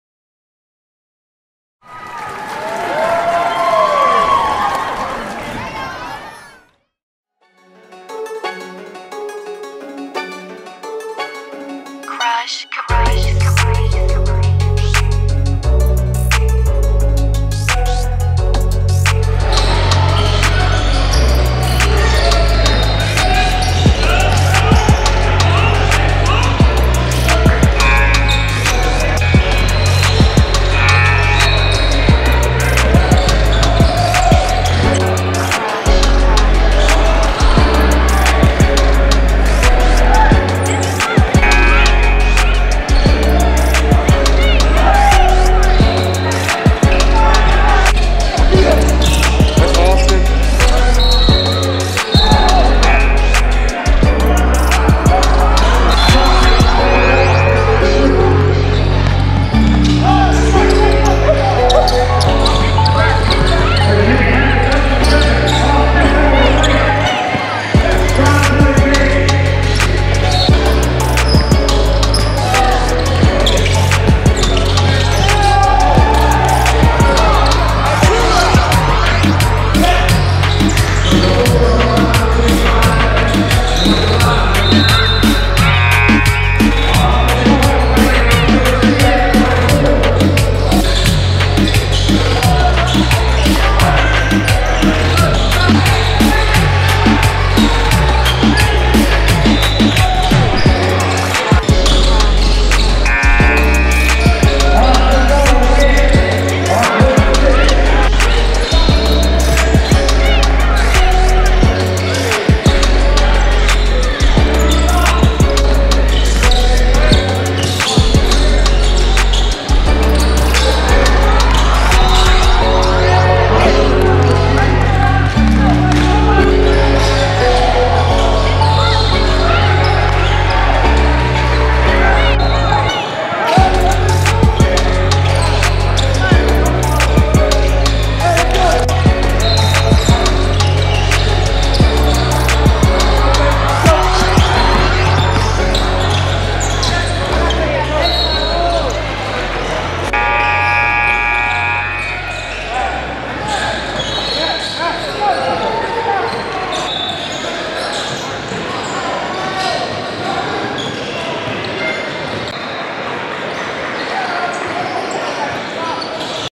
Recruiting Profile: 6'6 Austin Ball | Chapmanville Tigers | Full BIGSHOTS Highlights
RECRUITING PROFILE:
2022 6’6 W/G Austin Ball
with the Chapmanville Tigers
@ BIGSHOTS Rocky Top Tipoff

Filmed/edited by Jay Anderson.

Follow @BigShotsNation on Instagram and Twitter!